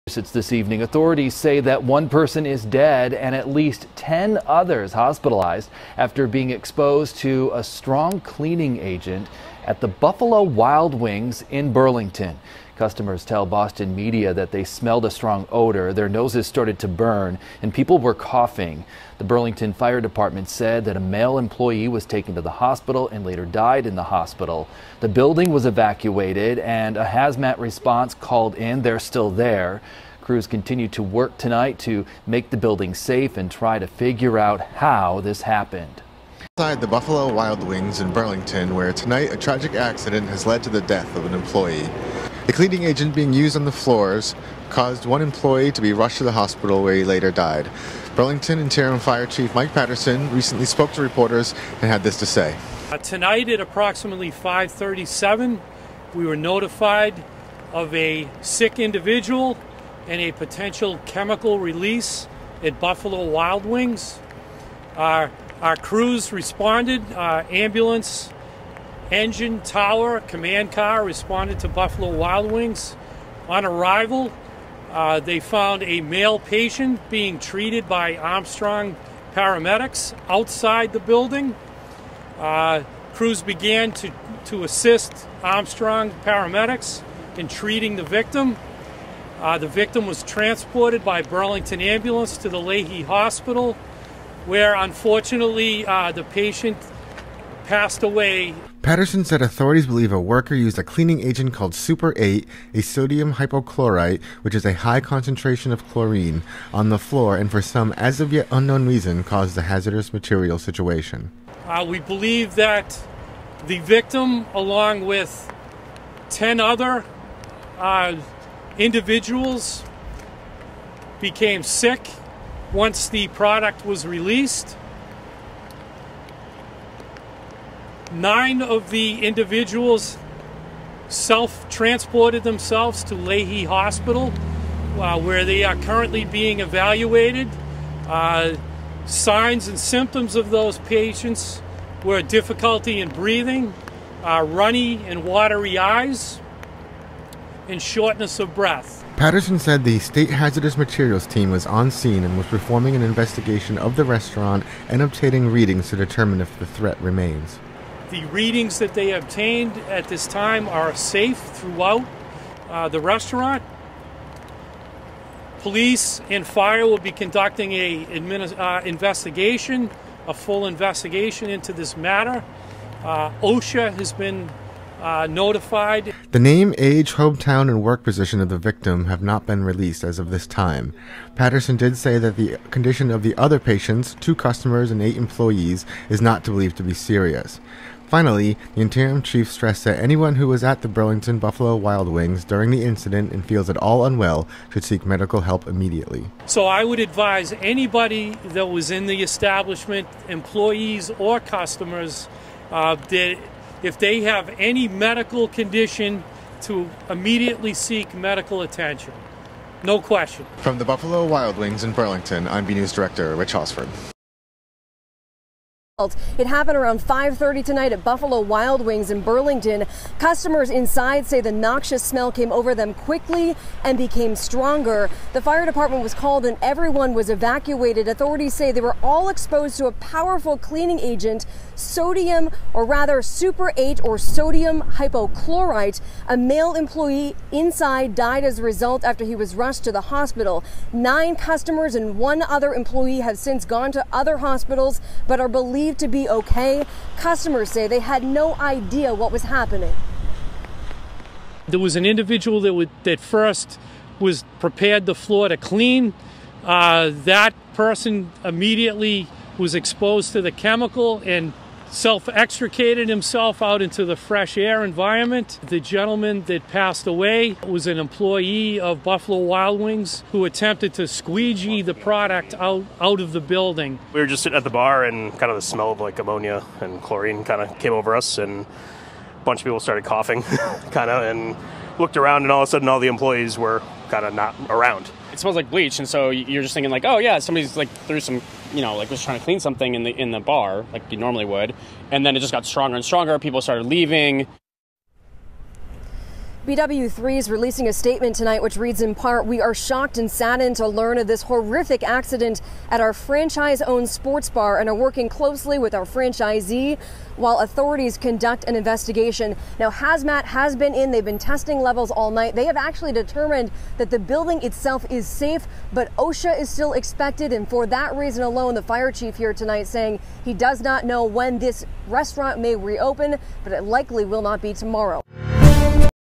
Buffalo Wild Wings Employee In Massachusetts Dies After Inhaling Strong Chemical
A Buffalo Wild Wings employee died and 10 other people were taken to the hospital Thursday night after being exposed to a cleaning agent in what authorities called a “serious hazmat material incident” at the restaurant.

The employee, a man, was trying to “squeegee” the chemical out of the kitchen area, when he apparently inhaled the substance and was taken to Lahey Hospital, where he died, Interim Fire Chief Michael Patterson said.

The cleaning agent, Super 8, contains a high concentrate of chlorine, Patterson said. He said it was unclear if that substance mixed with another chemical.